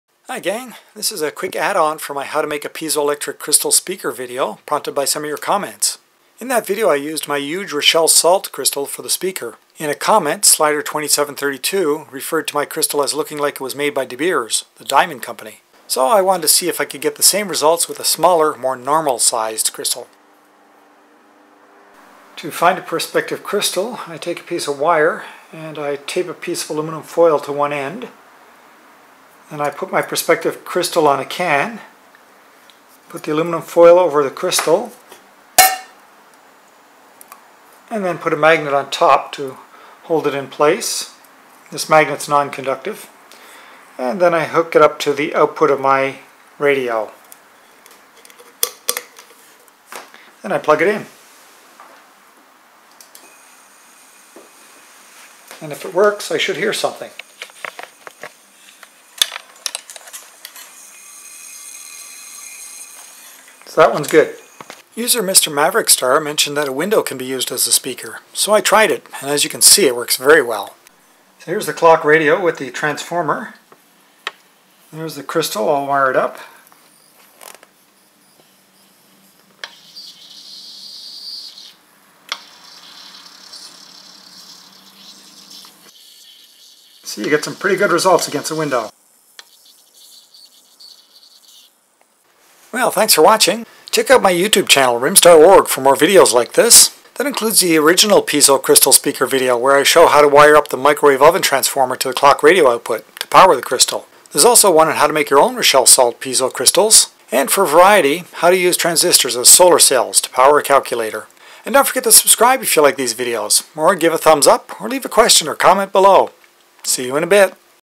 Piezoelectric Speaker - small crystals/window as speaker
More neat stuff with my piezoelectric crystal speaker using my homemade Rochelle salt crystals. One was to test a smaller crystal since that's what most people who make their own have, and the other was that these work great when pressed against a window.